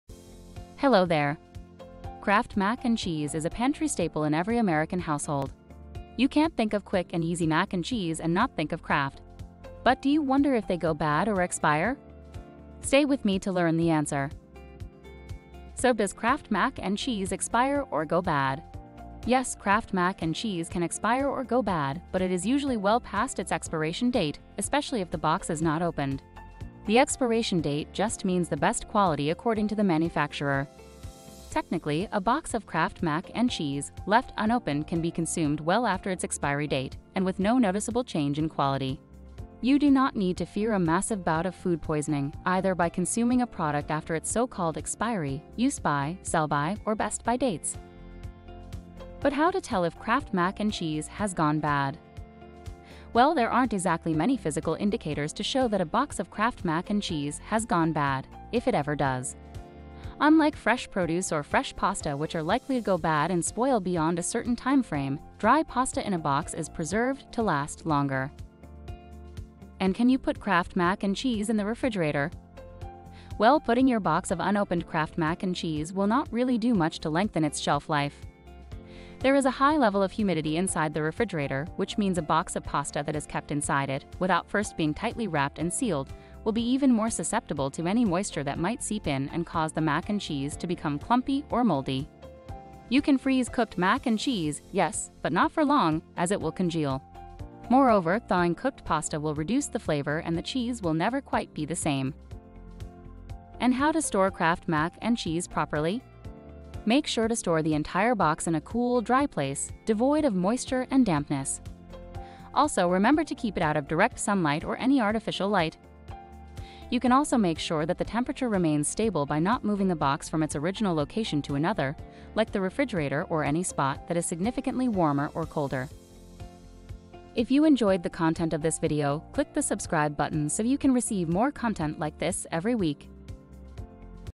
Does Kraft Mac and Cheese Go Bad or Expire? - Nutri Inspector
Does Kraft Mac and Cheese expire or go bad?

The answer to this question is not entirely straightforward. While expiration dates are typically indicative of when a food product will no longer be safe to consume, there are a number of factors that can influence the shelf life of Kraft Mac and Cheese. For example, the type of preservatives used in the product, as well as storage conditions, can impact how long the product will remain edible.

But How to Tell if Kraft Mac and Cheese Has Gone Bad?

If you're unsure as to whether your Kraft Mac and Cheese has gone bad, there are a few tell-tale signs you can look for. First, if the cheese sauce has separated into a watery liquid and chunky solid, it's probably time to toss it. secondly, if the pasta is mushy or gummy, it's also a sign that it's gone bad.

How to Store Kraft Mac and Cheese Properly?

To store Kraft Mac and Cheese properly, you should first remove it from the packaging. Then, place it in an airtight container or bag. If you are using a container, you should put the mac and cheese in the fridge. If you are using a bag, you can store the mac and cheese in the freezer.

for more interesting stories.